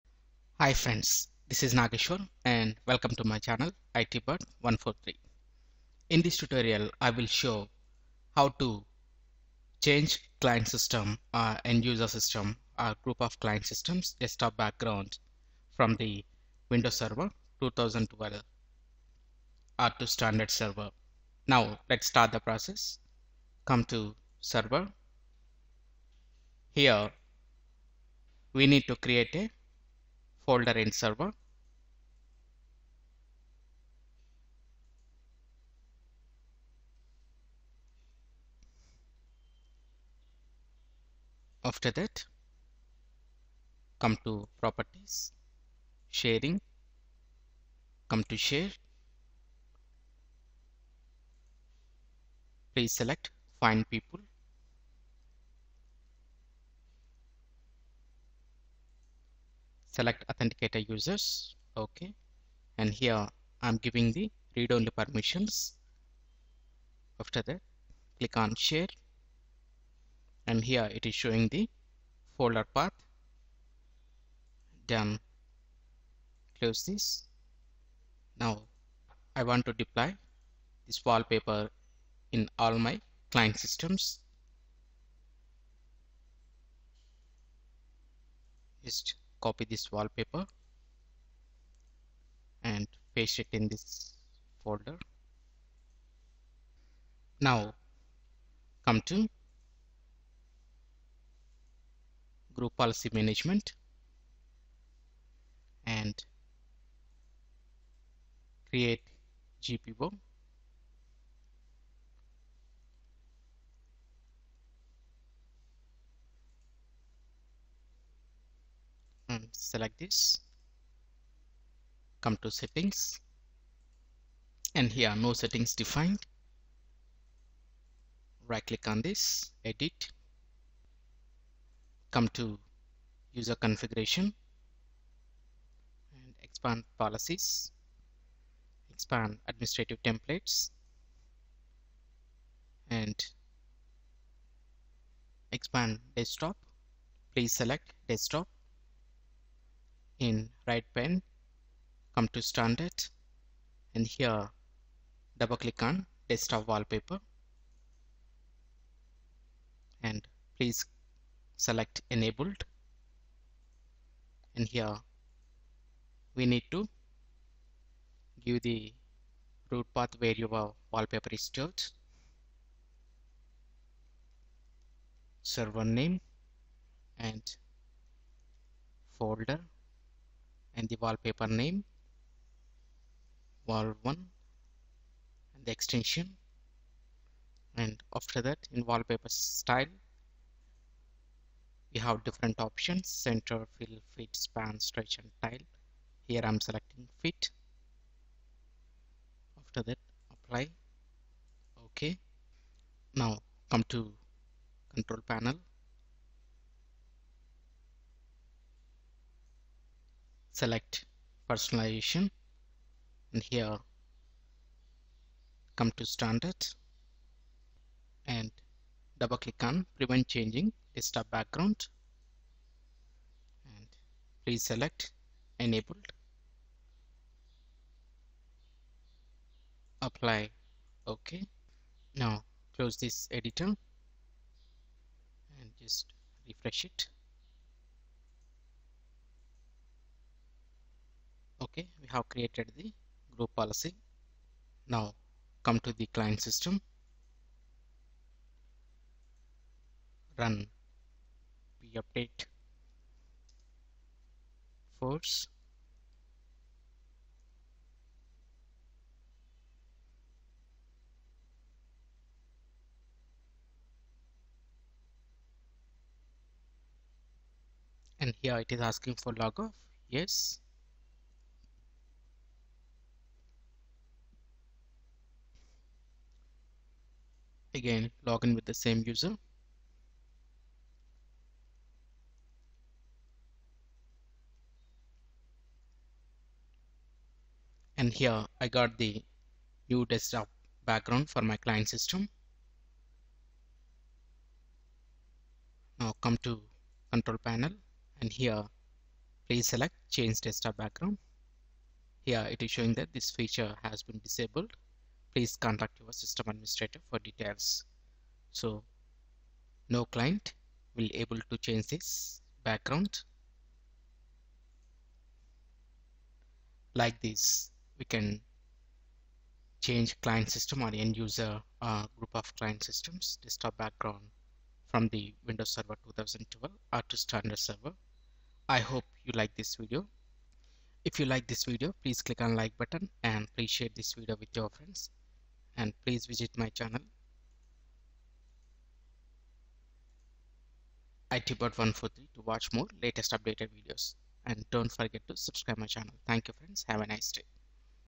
how to deploy desktop wallpaper through group Policy in windows server 2012 r2
In This tutorial, I have shown how to deploy desktop wallpaper on client systems through group policy in windows server 2012 r2.